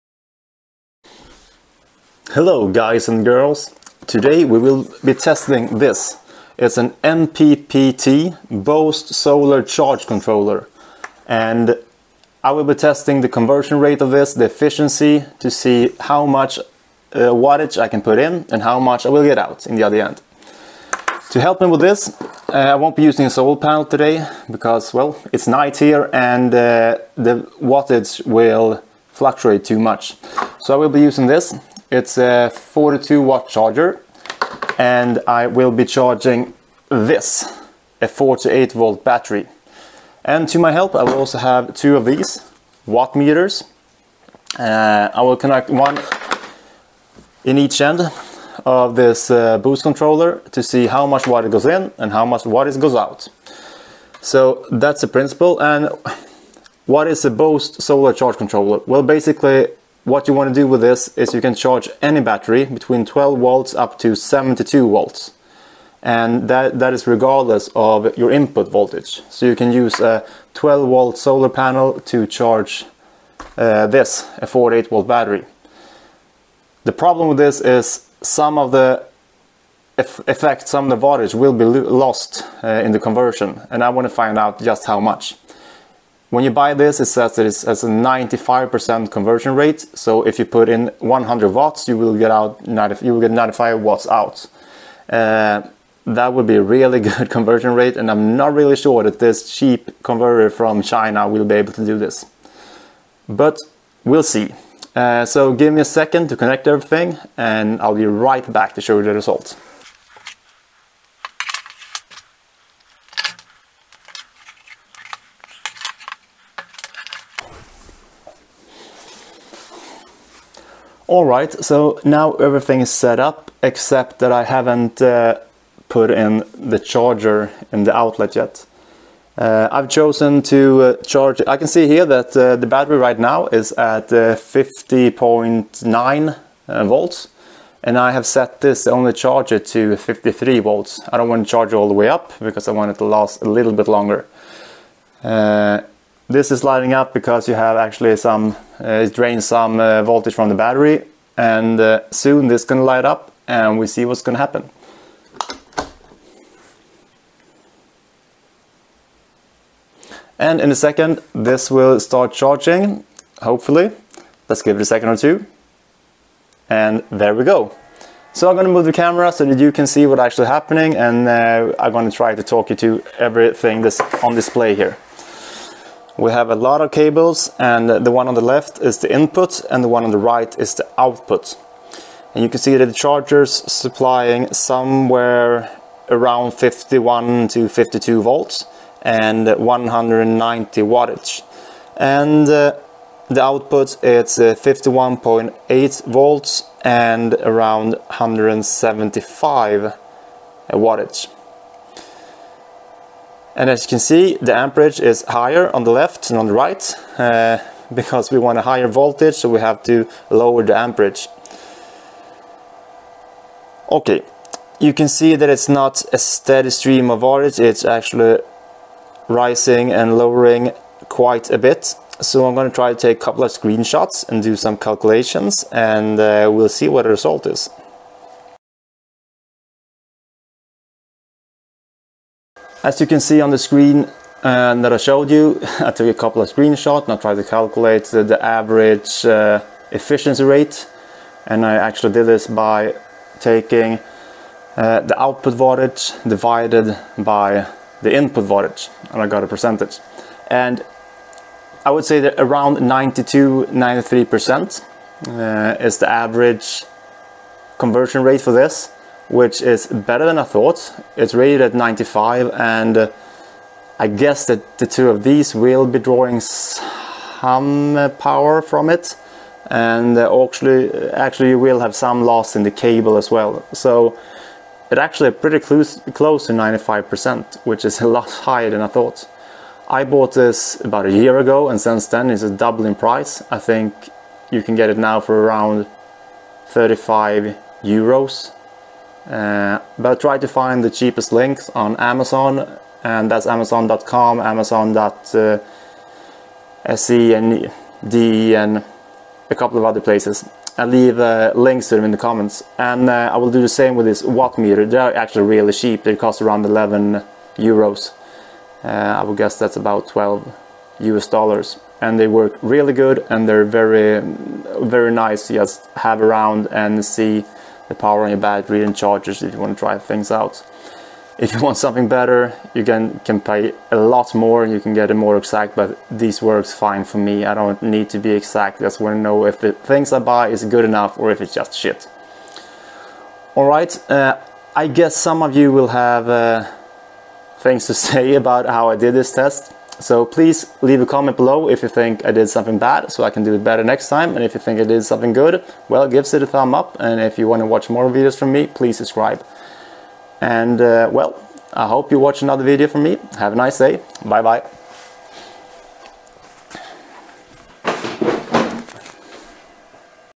MPPT Boost solar charge controller 12v-72v
What is the actual efficiency of the cheapest boost solar charge controller on the market? Will it deliver on the 95% conversion rate from the product description?

🆓 Free Amazon Prime 30 days!

🛒 MPPT boost solar charge controller.

🍺🍺🍺 If you would like to donate a beer please visit the link below 🍺🍺🍺